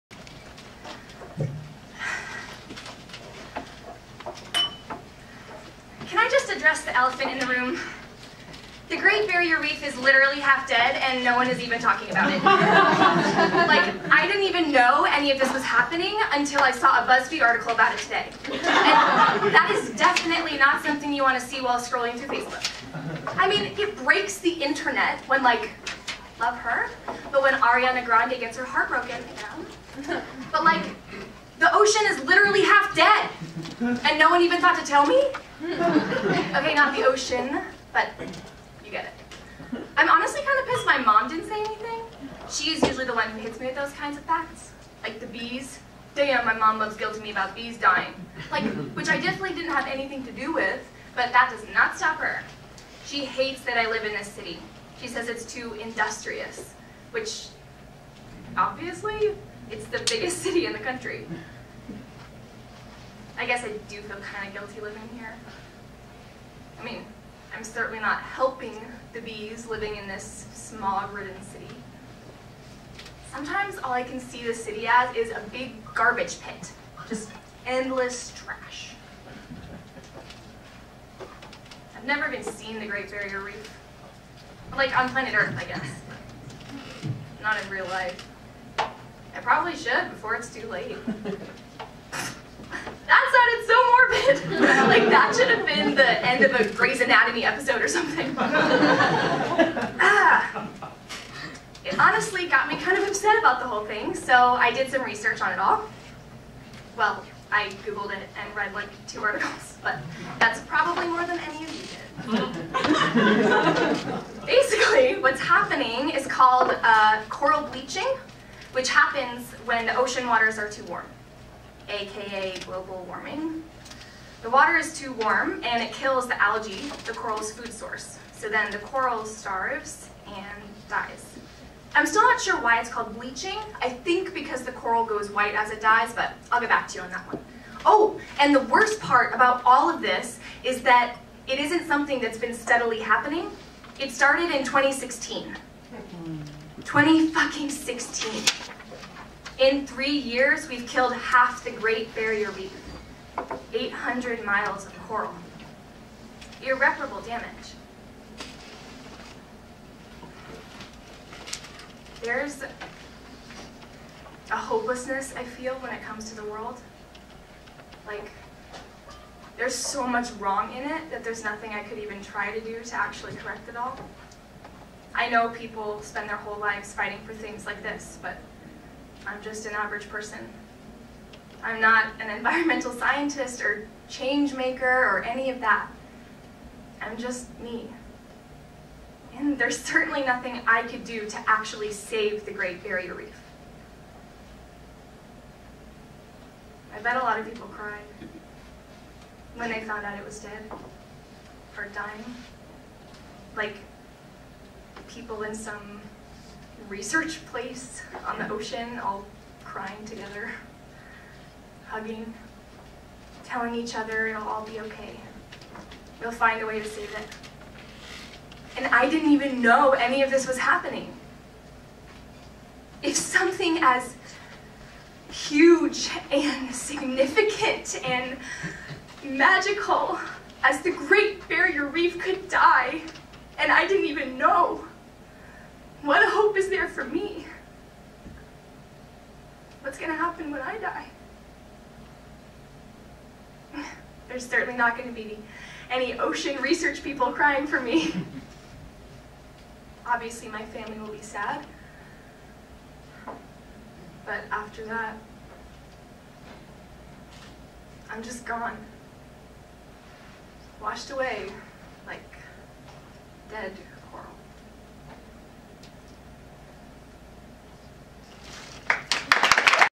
"From Inside...the Great Barrier Reef" Monologue
Monologue from play "From Inside..." by Zach Decker
Performed 2018 at The Green Room 42.